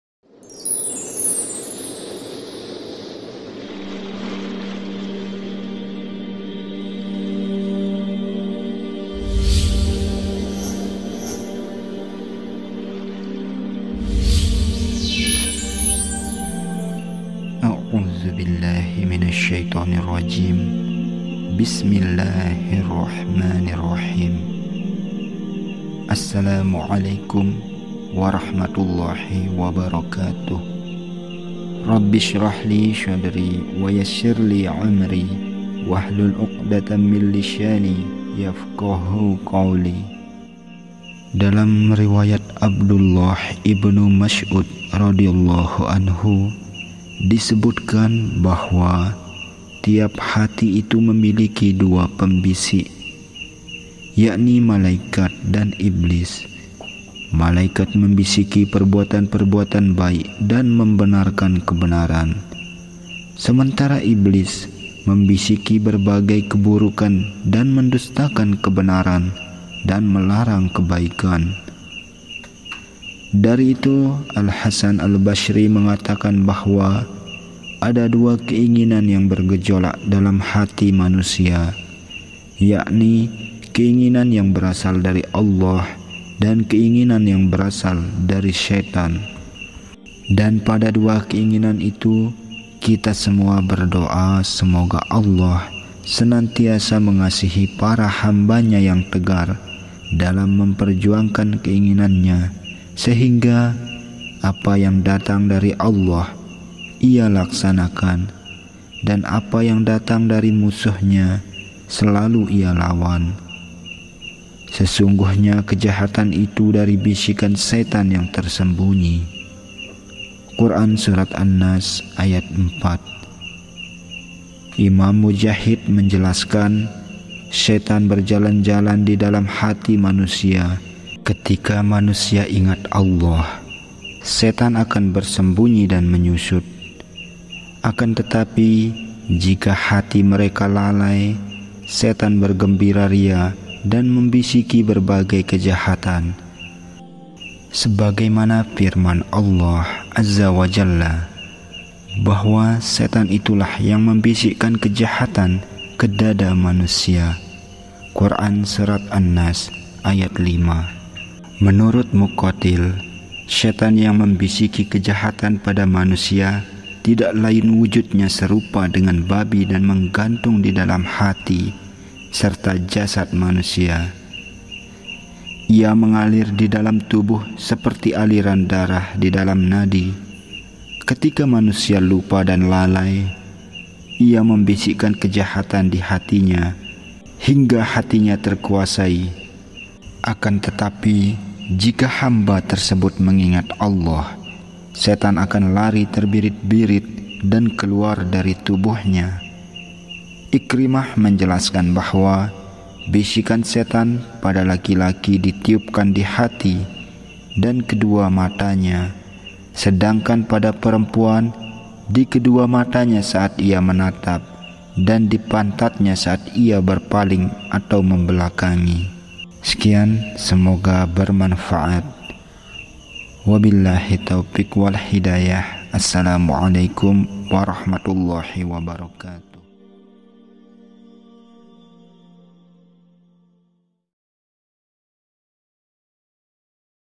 Bisikan Hati Pada Laki-Laki & Perempuan
Bisikan setan pada laki-laki ditiupkan di hati dan kedua matanya, se dangkan pada perempuan di kedua matanya saat ia menatap, dan di pantatnya, saat ia berpaling (mem belakangi).

Sumber : Tafsir Al-Jailani